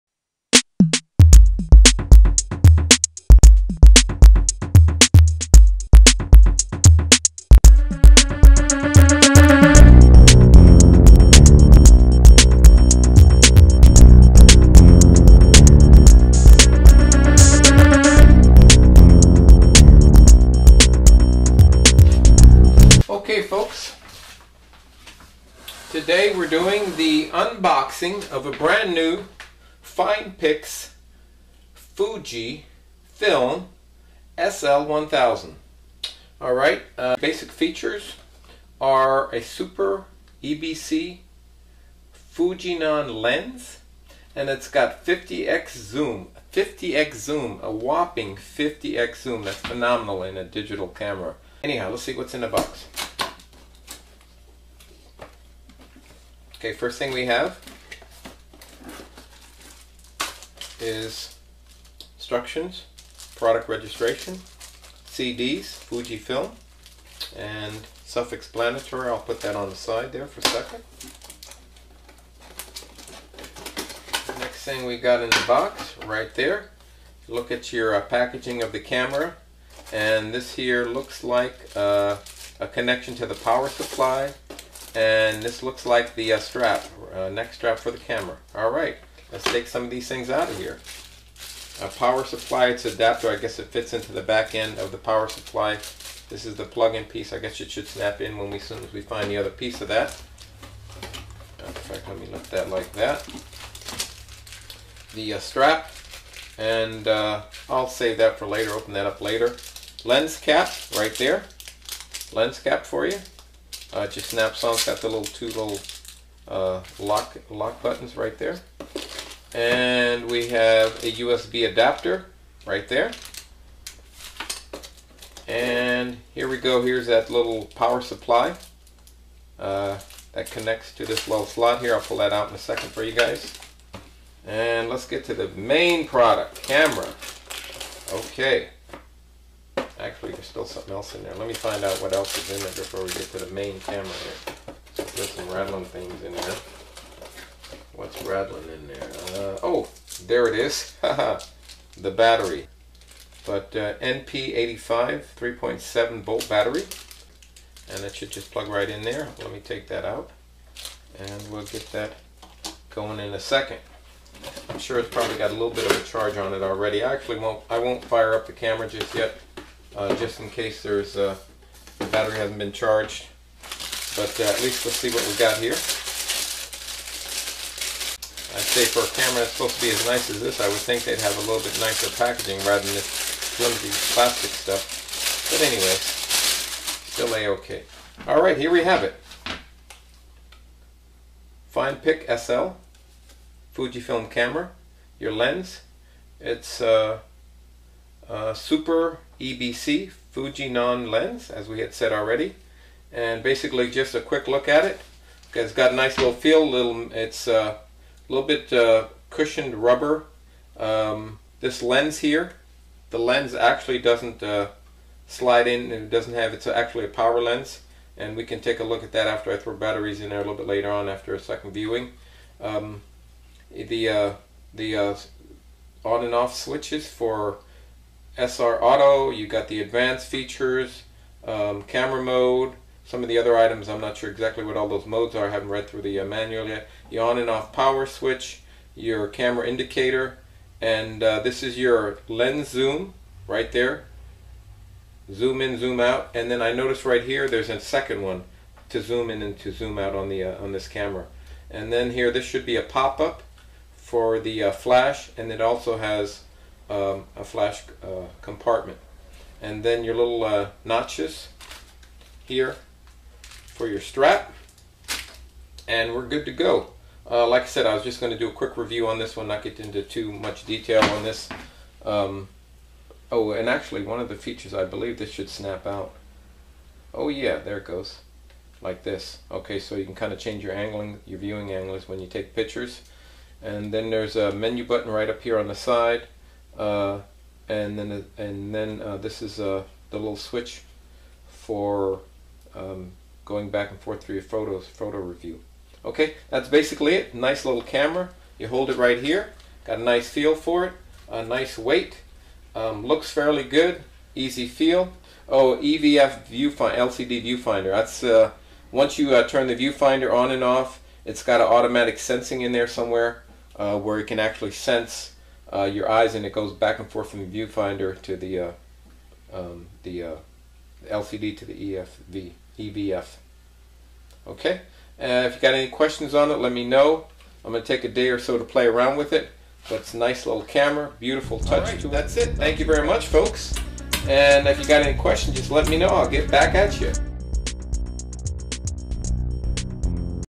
Fujifilm Finepix SL1000 Unboxing/Review
Unboxing of the new FujiFilm Finepix SL1000 Camera with 50 x zoom lens. Feedback and comments are allowed for now. But please keep them respectful, otherwise comments will be disabled.